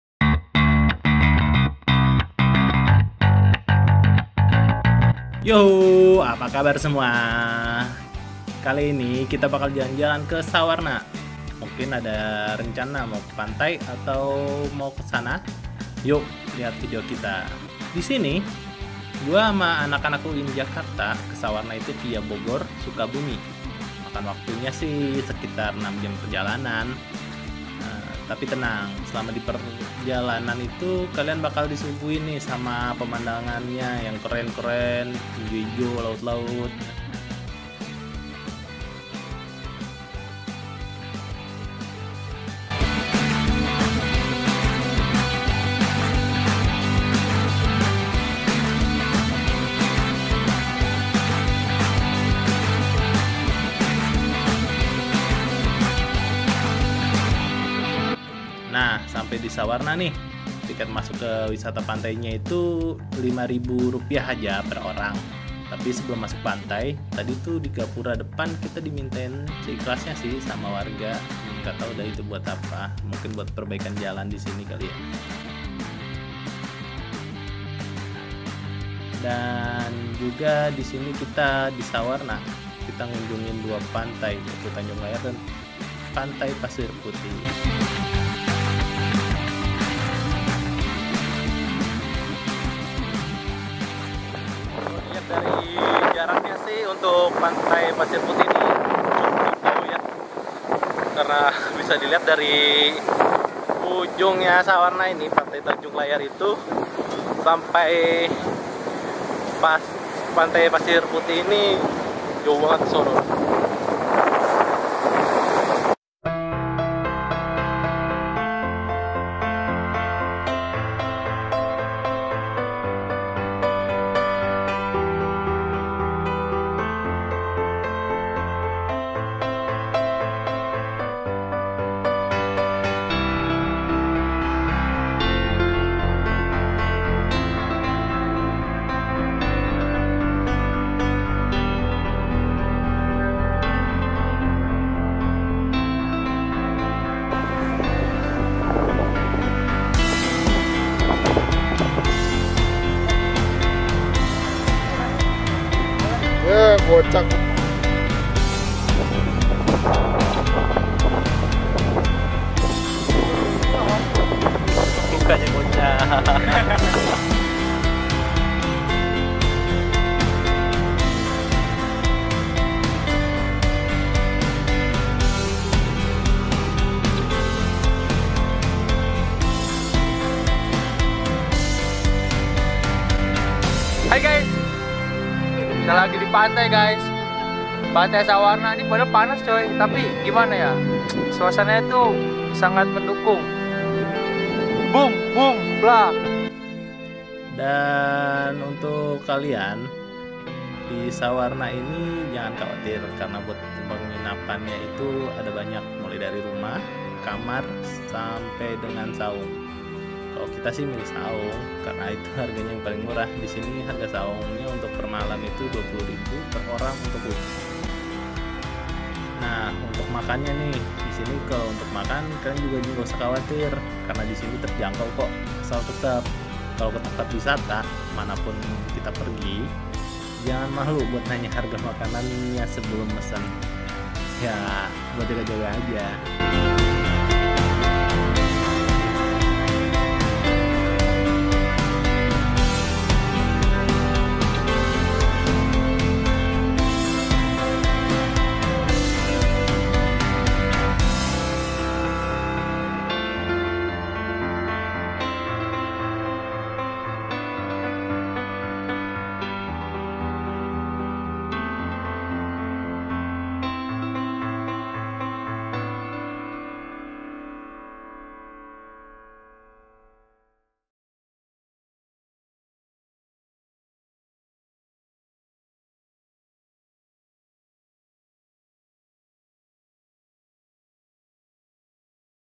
Sawarna Jalan-jalan Cuman 100rb-an
Yang mau jalan-jalan ke pantai yuk cek video kita ke sawrna cuman modal 100rb bisa nikmatin pantai yg asik yuk